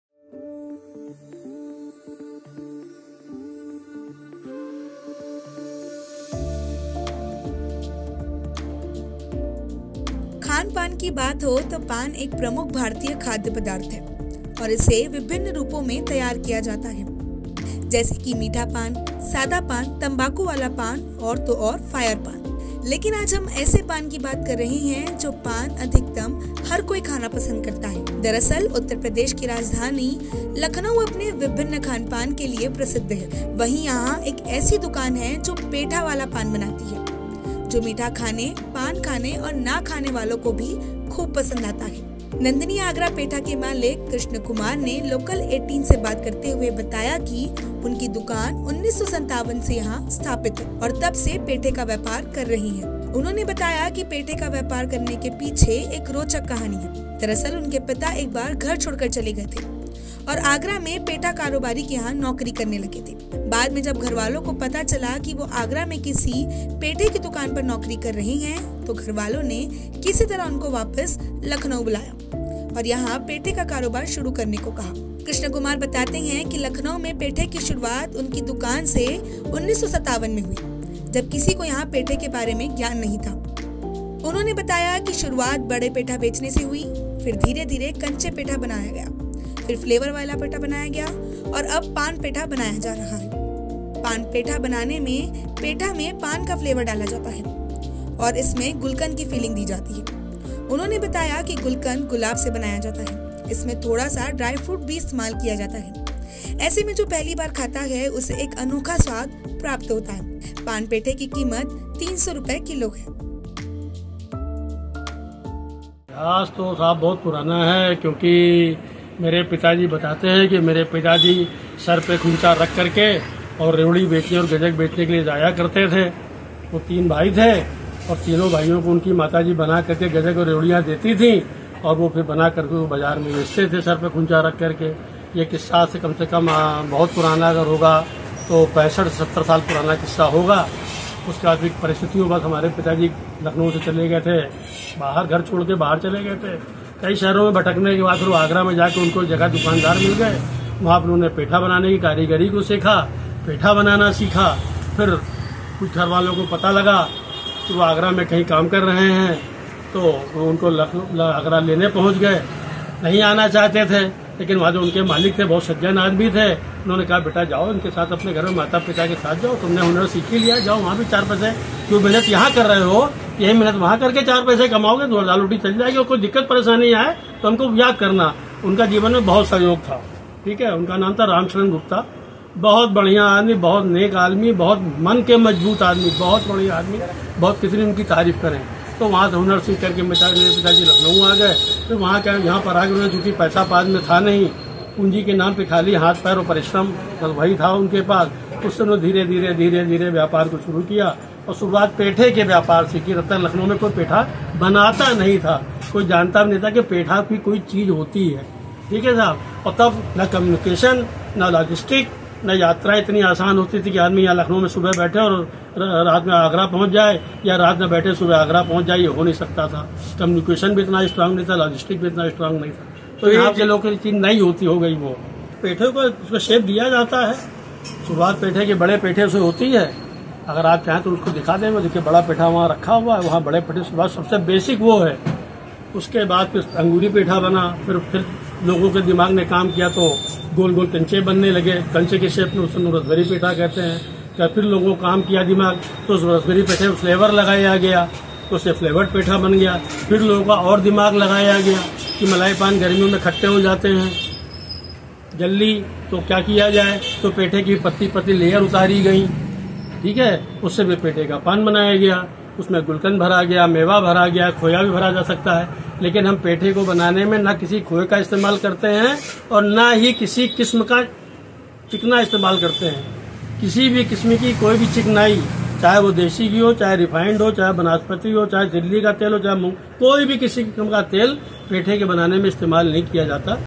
Sweet Dish : सालों पुरानी इस दुकान के पान पेठा हर किसी को है भाता
Lucknow News : खानपान की बात हो तो पान एक प्रमुख भारतीय खाद्य पदार्थ है और इसे विभिन्न रूपों में तैयार किया जाता है. जैसे की मीठा पान, सादा पान, तंबाकू वाला पान और तो और फायर पान, लेकिन आज हम ऐसे पान की बात कर रहे हैं जो पान अधिकतम हर कोई खाना पसंद करता है. दरअसल उत्तर प्रदेश की राजधानी लखनऊ अपने विभिन्न खानपान के लिए प्रसिद्ध है.
Lucknow News: Talking about food, Paan is a major Indian food item and it is prepared in different forms. Like sweet paan, plain paan, tobacco paan and even fire paan, but today we are talking about such paan which everyone likes to eat the most. Actually, Lucknow, the capital of Uttar Pradesh, is famous for its various food.